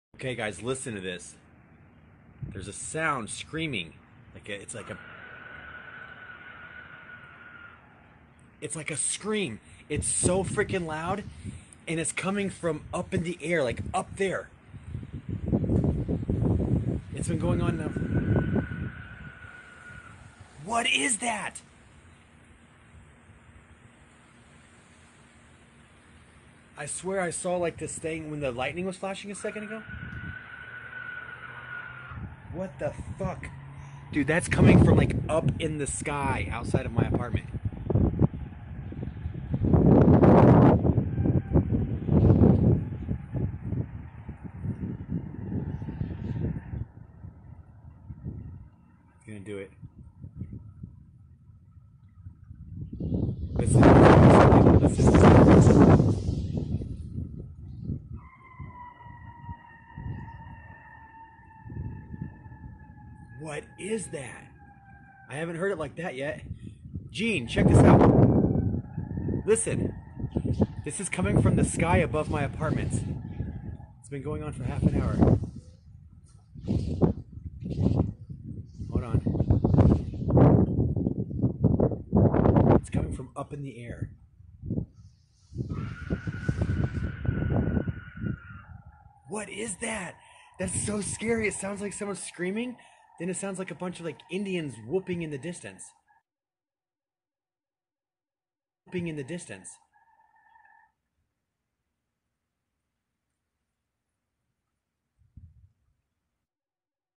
Bryan Texas "Screams" In The Sky ?? Lightning Storm May 31 2020
wtf creepy noises heard in Bryan Texas with lightning up in the sky creepystories anyone know what this is?